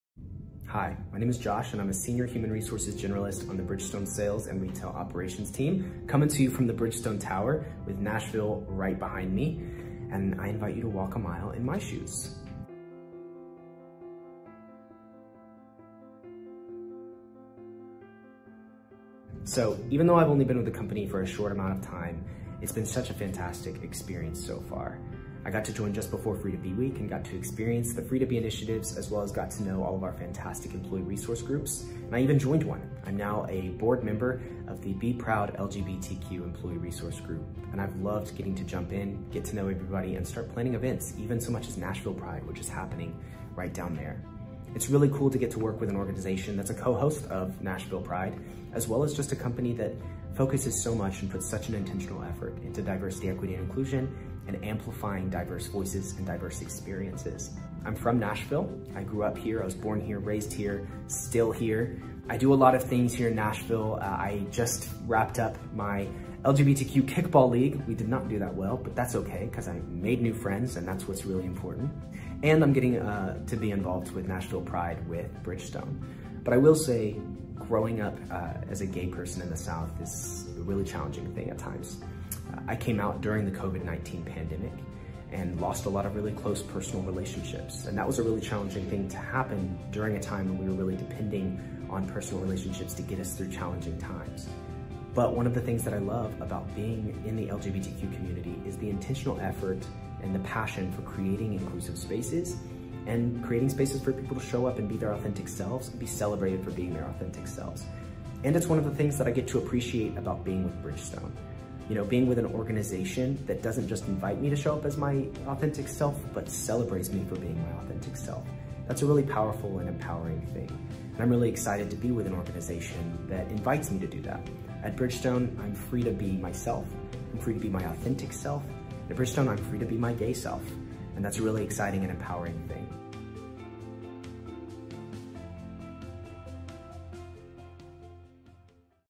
Walk a Mile in My Shoes Josh Stout
Josh Stout is a Sr. HR Generalist, a board member for our LGBTQ+ employee resource group, and a Nashville native.  Walk a mile in Josh’s shoes to learn what else he is Free to Be at Bridgestone.